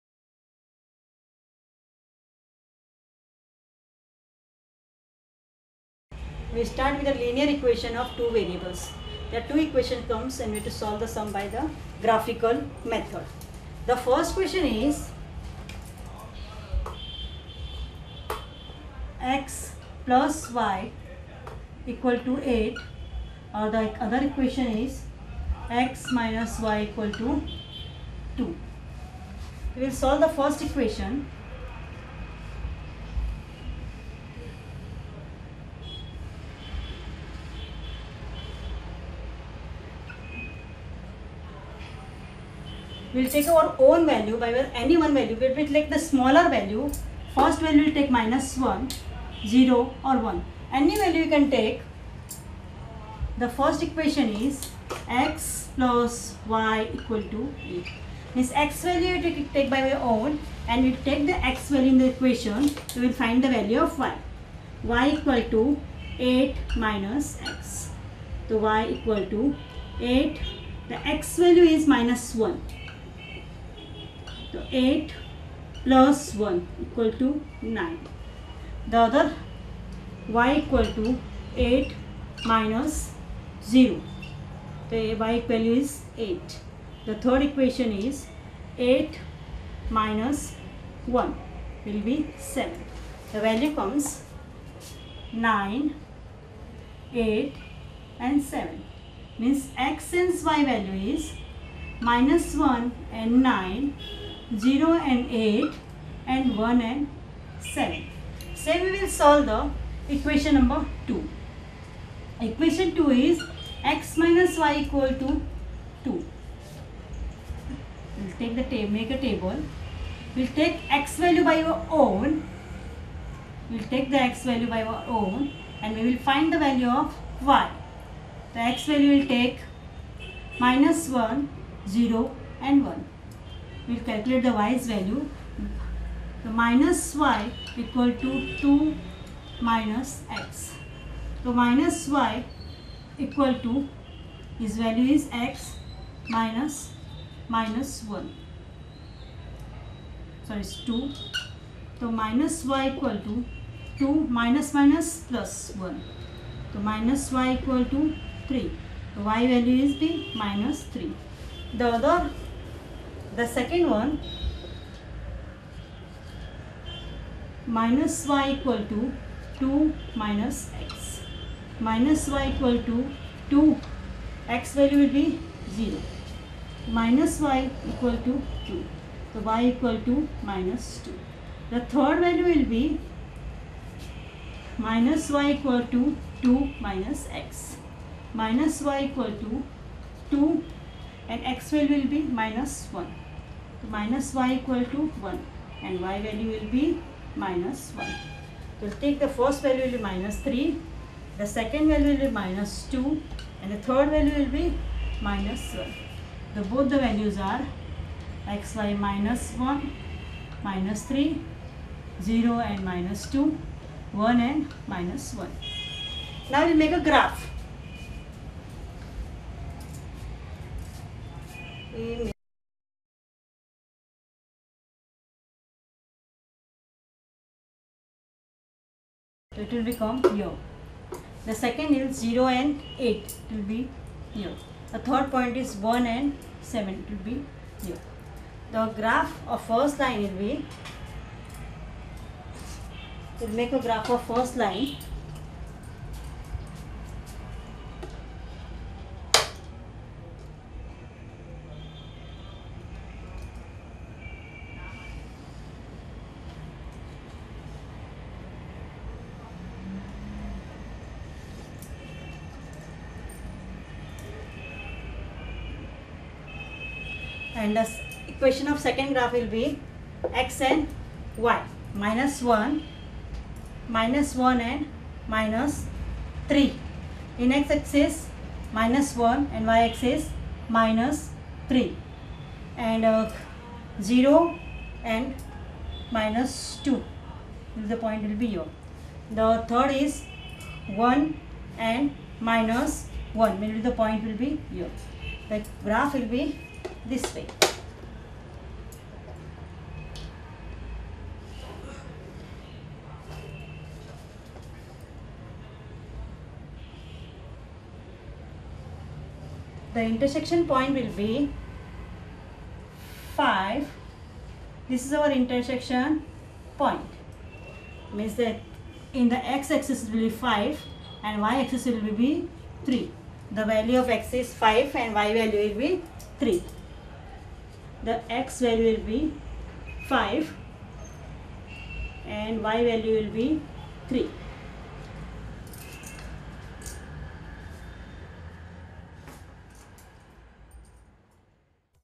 10th Std Algebra, Chp.3 Linear Equations in Two Variables - Ex.3.1 Q.1
SSC (10th STD) Algebra Subject, Maharashtra Board, Chapter 3: Linear Equations in Two Variables, Exercise 3.1, Question number 1.
Updated Syllabus.